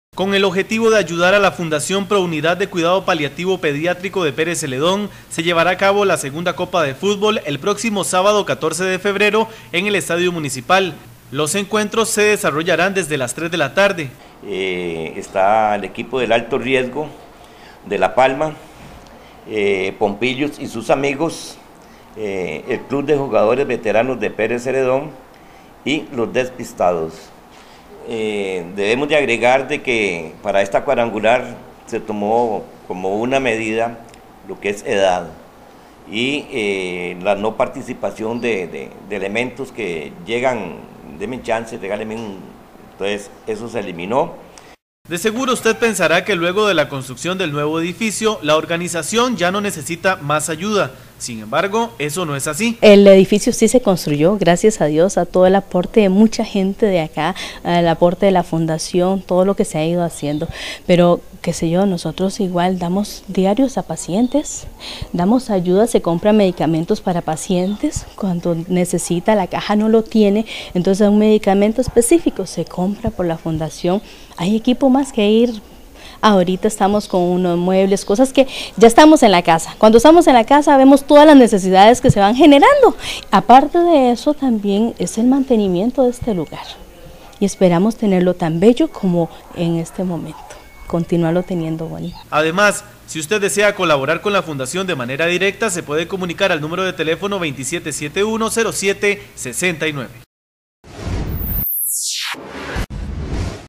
Cuidados Paliativos realizará su segunda edición de la Copa Rotativa
Con el objetivo de ayudar a la Fundación Pro Unidad de Cuidado Paliativo Pediátrico de Pérez Zeledón, se llevará a cabo la segunda copa de futbol, el próximo sábado 14 de febrero en el Estadio Municipal de Pérez Zeledón.